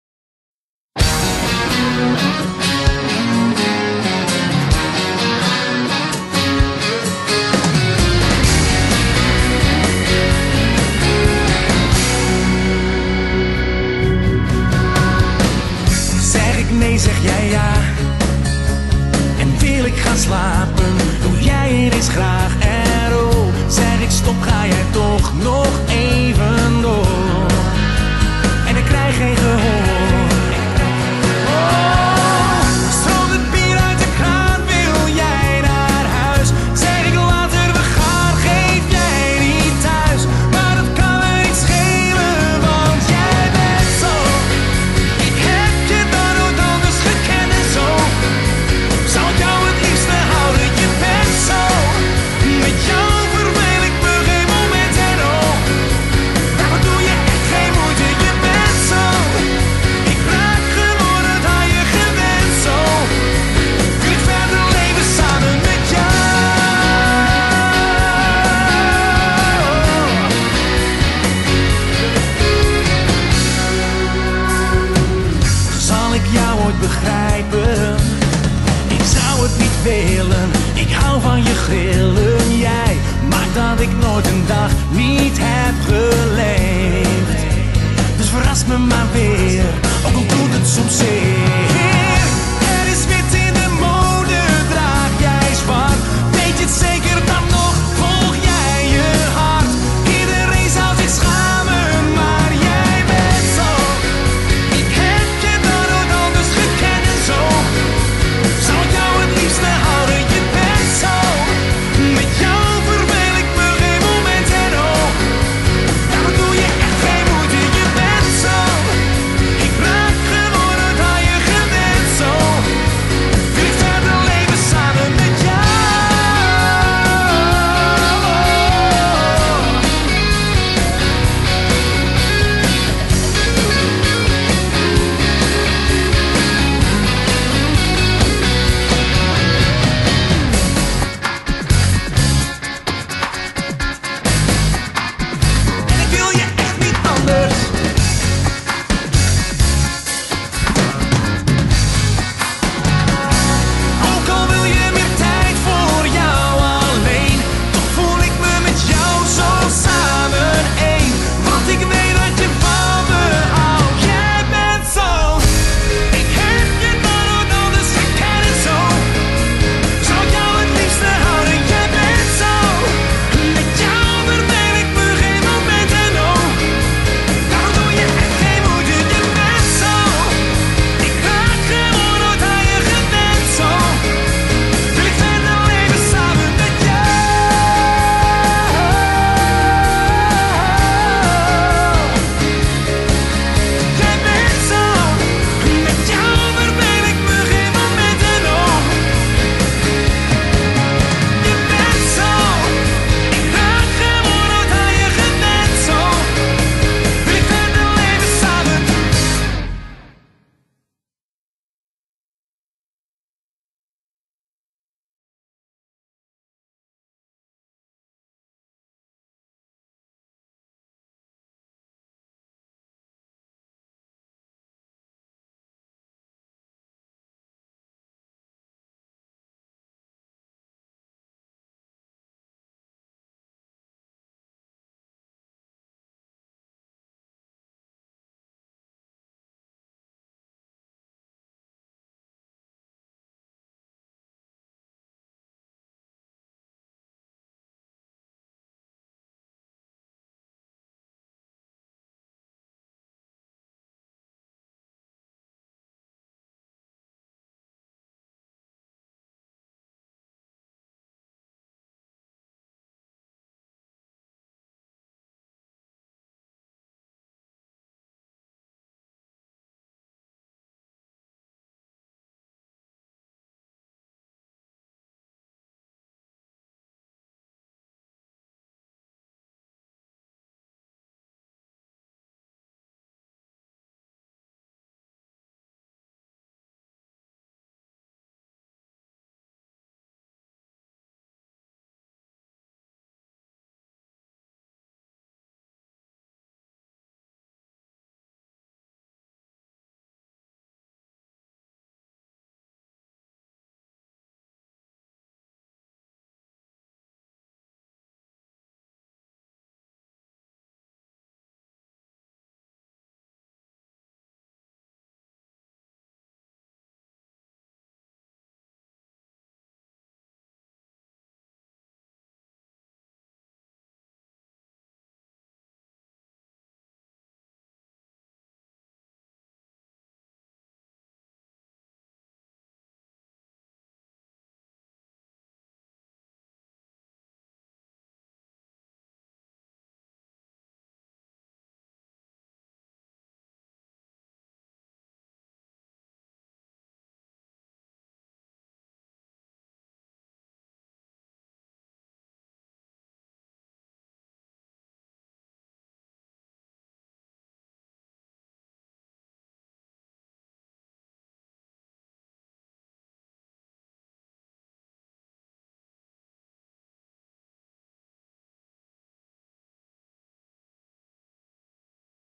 Gymnasium Bernrode - met de Kunstbus naar Amsterdam
10 december, Gymnasium Bernrode gaat naar Amsterdam.
Hier een fotoreportage van één van die groepjes.